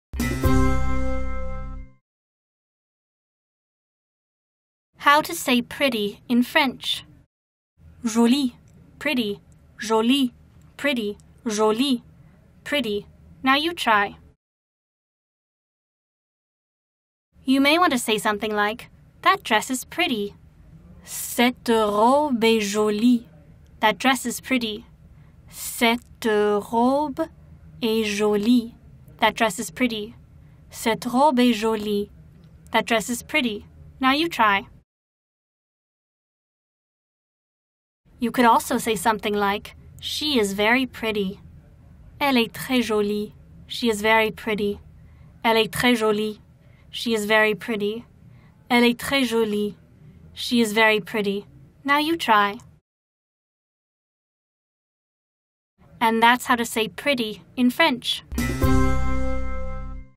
How to Say "Pretty" in French | French Lessons
How to say pretty in French.
Joli.
Pretty.
Joli.
Pretty.
Joli.
Pretty.
Now you try.
You may want to say something like, that dress is pretty.
Cette robe est jolie.
That dress is pretty.
Now you try.
You could also say something like, she is very pretty.
Elle est très jolie.
She is very pretty.
Now you try.
And that's how to say pretty in French.